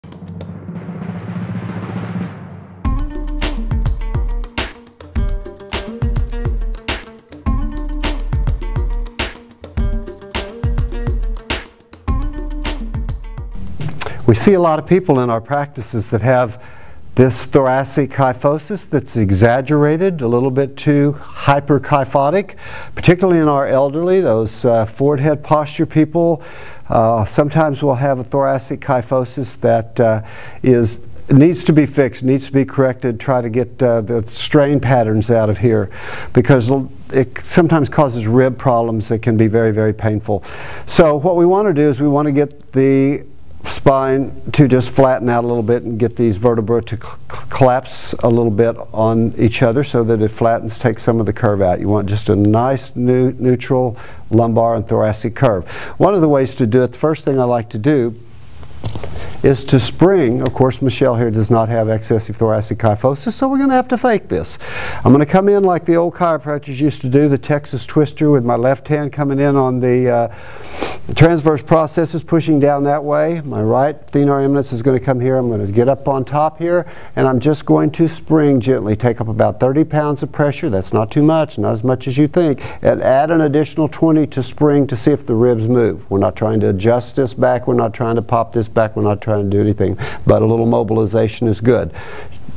Hyper and Hypo Kyphosis - Erik Dalton
Help clients, boost referrals, advance your skills. to check out our monthly specials

Improve posture, function, and back pain with unique myoskeletal alignment techniques by Erik Dalton. Add this deep tissue fascial treatment to your massage and bodywork practice.